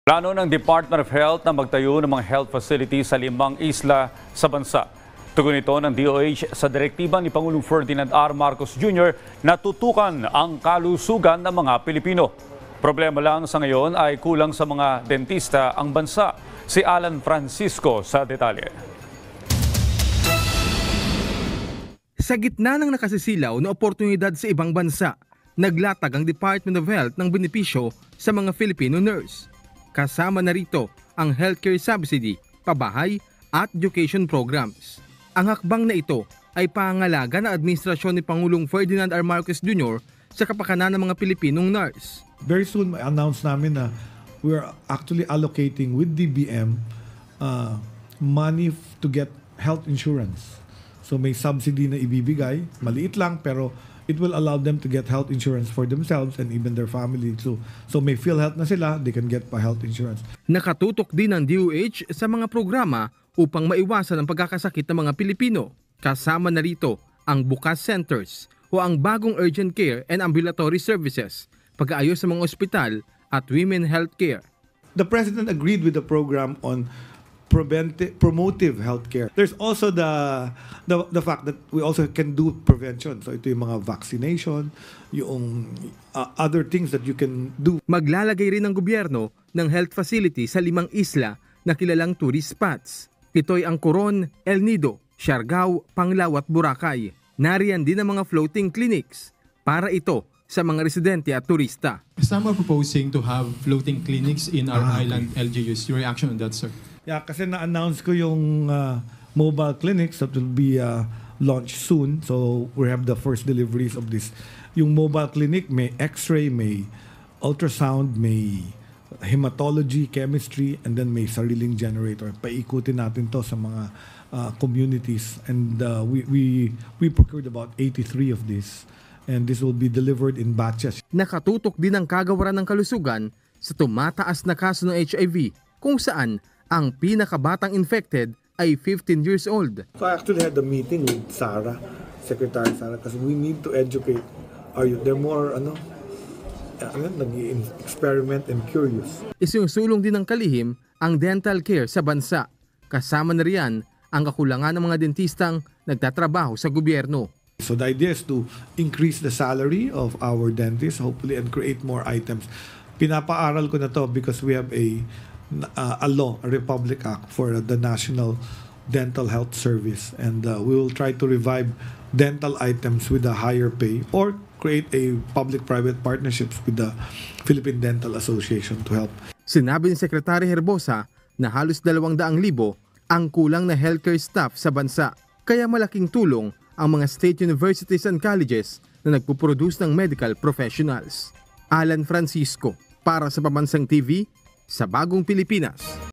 Healthcare subsidy, pabahay, education programs, at iba pang benepisyo para sa Pinoy nurses....
Healthcare subsidy, pabahay, education programs, at iba pang benepisyo para sa Pinoy nurses, inilatag;

DOH, nakatutok din sa mga programa para maiwasan ang pagkakasakit ng mga Pilipino